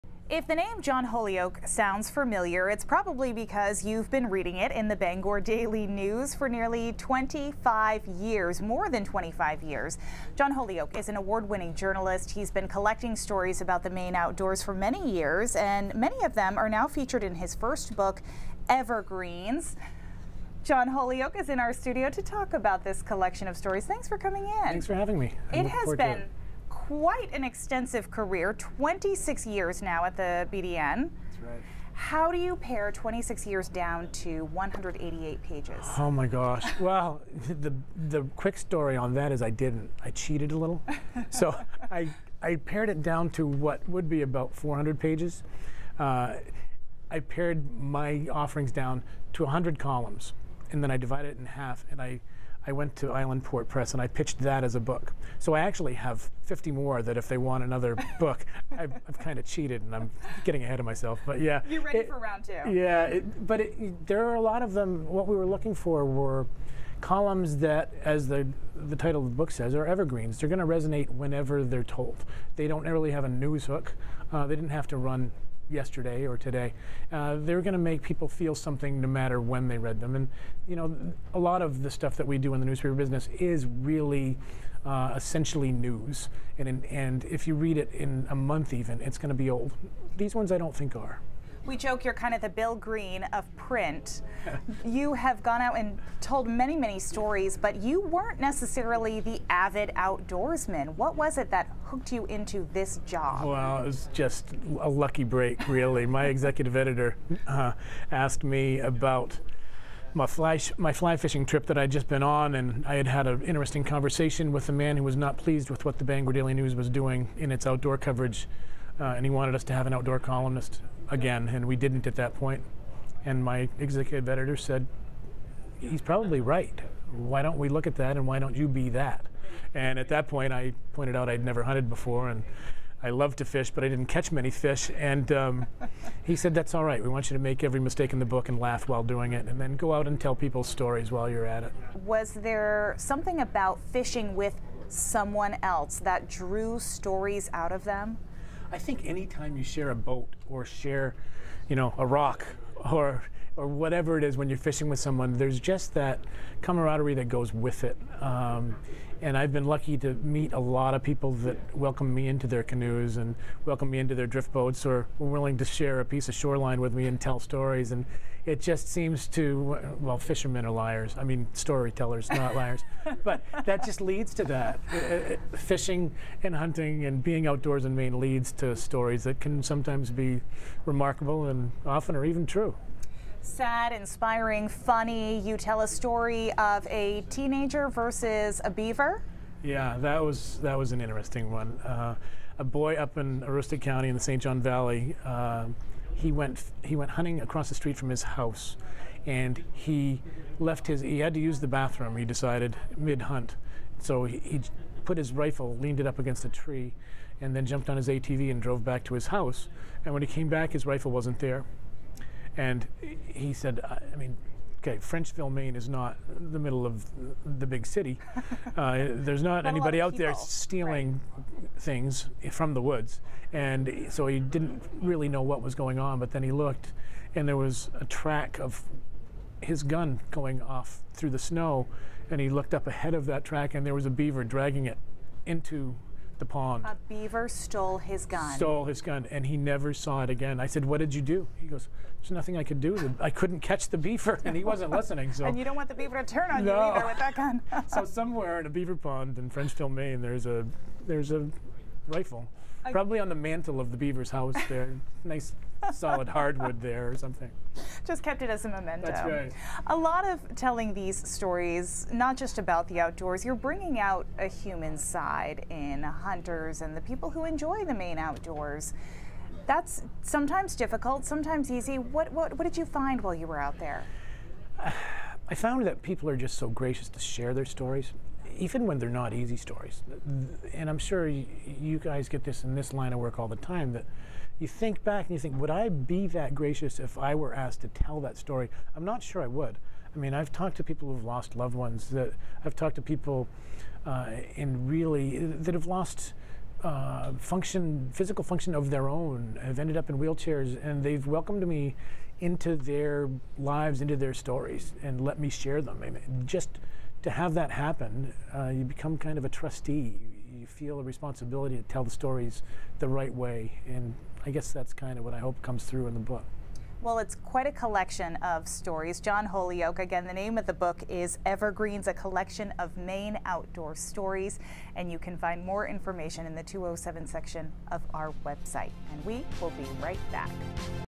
New Book tells stories of the Maine Outdoors
John Holyoke compile some of his favorite stories from the Maine outdoors.